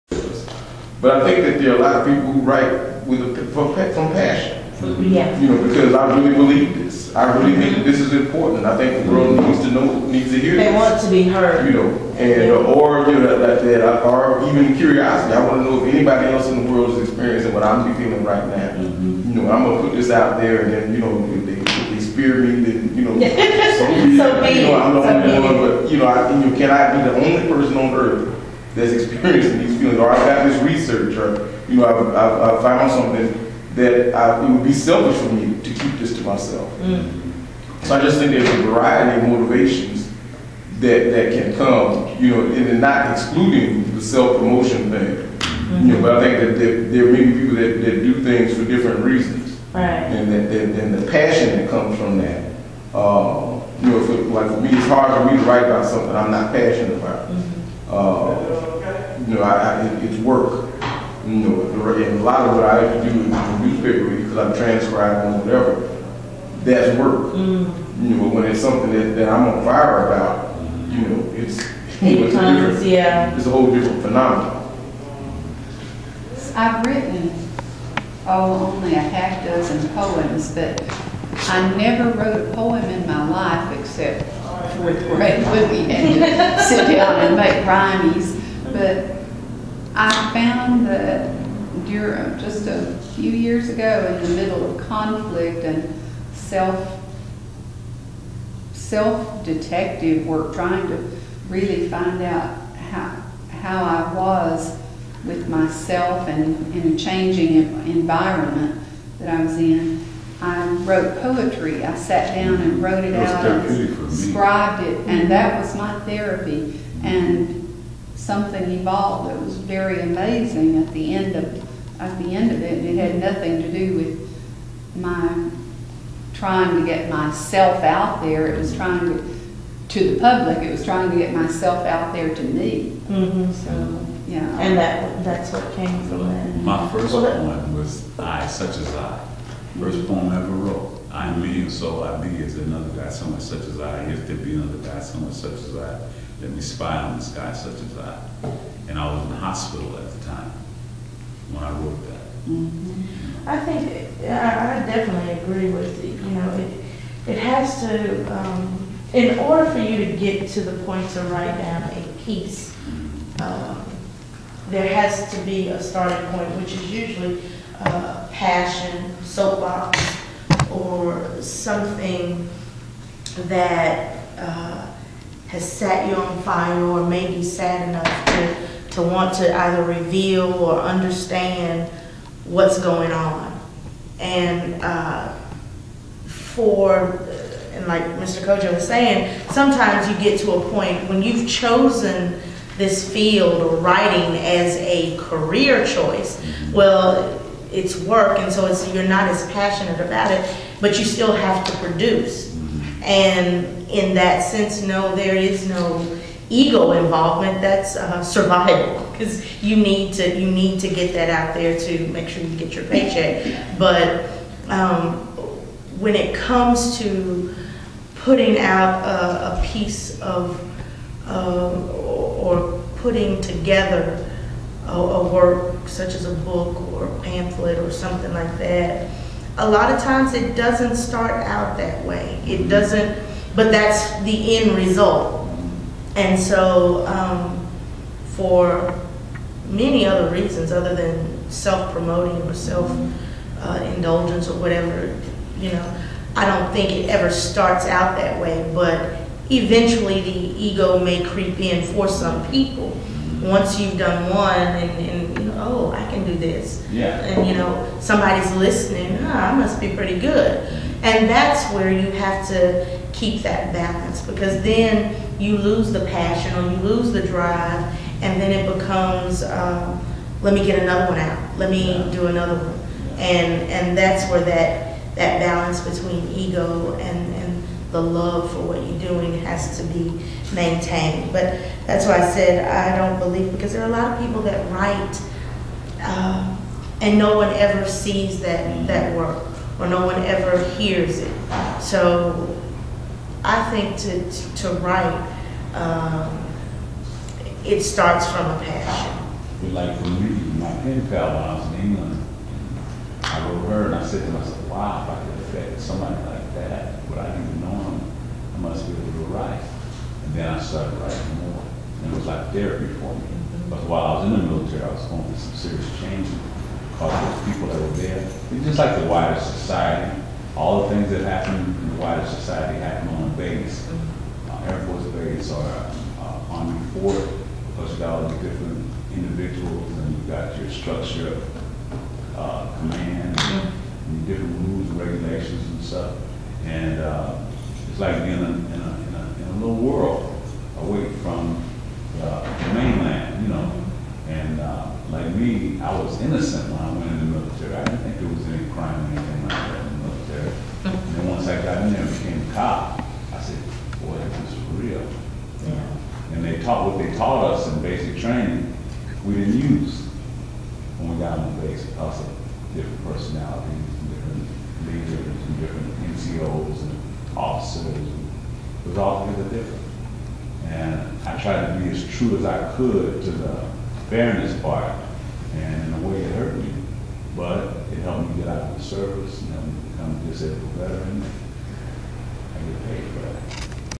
Authors Market Jan 008.MP4
The Author's Market is Networking program designed to help local authors with books, gain exposure, ask questions they may have and re-introduce themselves to the public with other like-minded individuals. This open forum also aids author enthusiast.